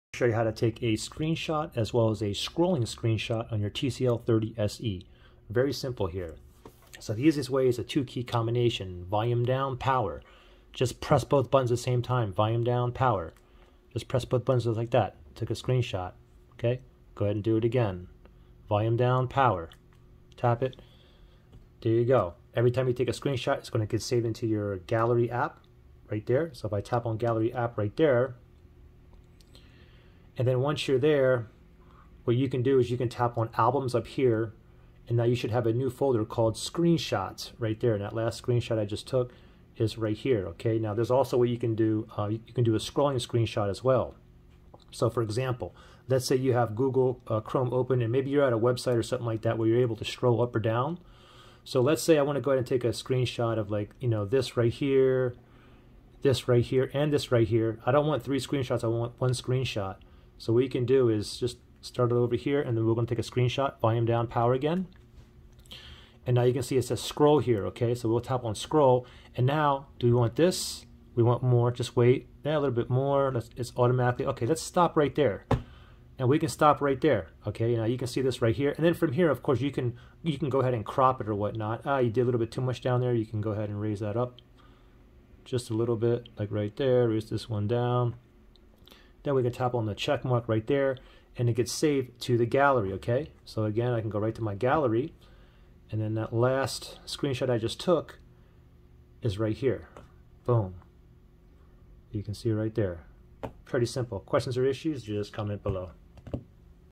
TCL 30SE/30XL/30/30E etc: How to Take Screenshot & Scrolling Screenshot
I show you how to take a screenshot and scrolling screenshot on the TCL 30 SE Smartphone. This should also work on the TCL 30, TCL 30 XL, TCL 30+, TCL 306, TCL 305, TCL 30 V, TCL 30E).

TCL 30 SE |2022| 6.52'' Unlocked Cell Phone, 4+64GB Android Phone GSM Unlocked